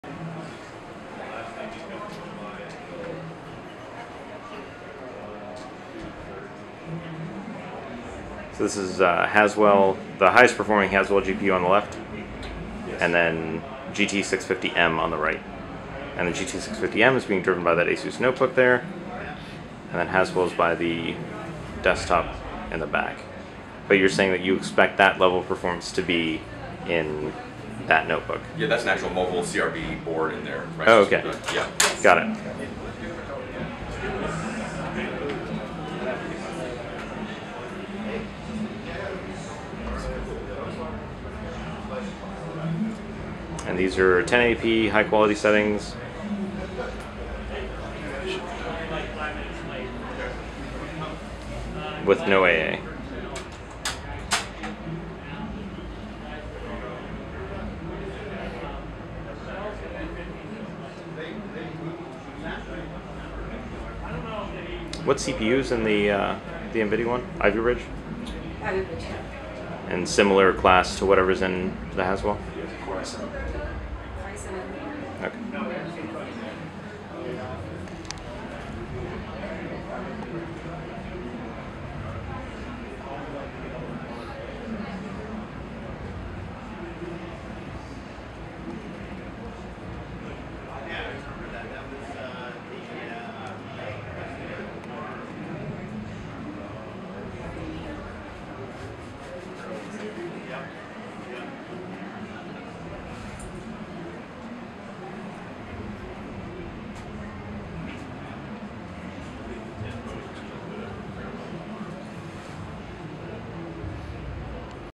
Haswell vs. NVIDIA GeForce GT 650M Performance Demo
Intel's Haswell GT3e mobile reference motherboard in a desktop chassis compared to an ASUS UX15 with on-board GeForce GT 650M.